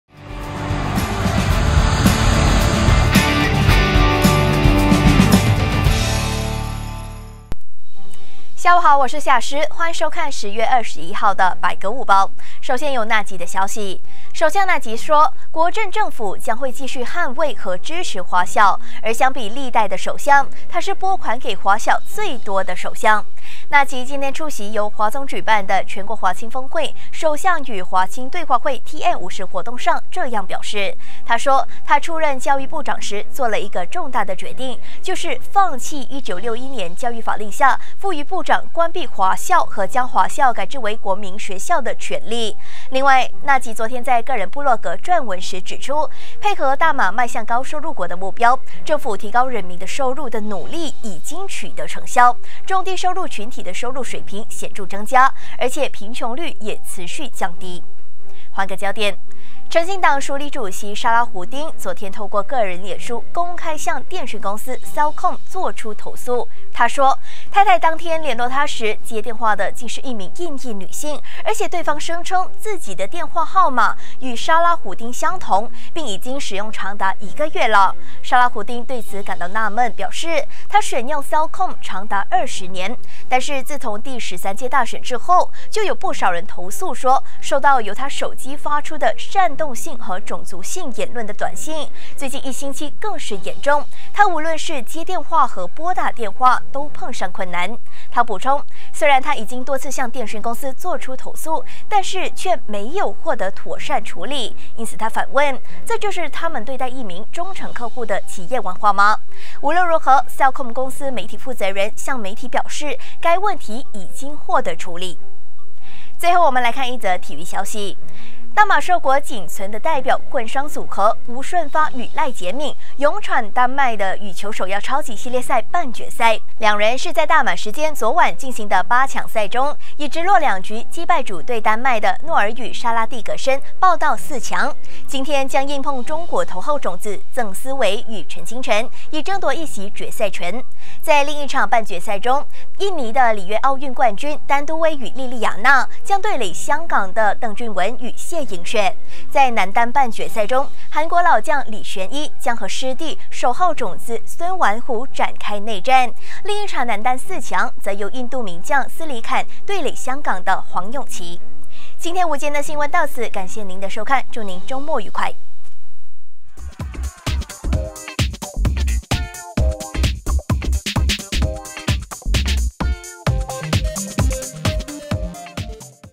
沙拉胡丁投诉：我和印裔女性同个电话号码
诚信党署理主席沙拉胡丁透过个人脸书公开向电讯公司Celcom作出投诉，声称太太联络他时，接电话的竟是一名印裔女性！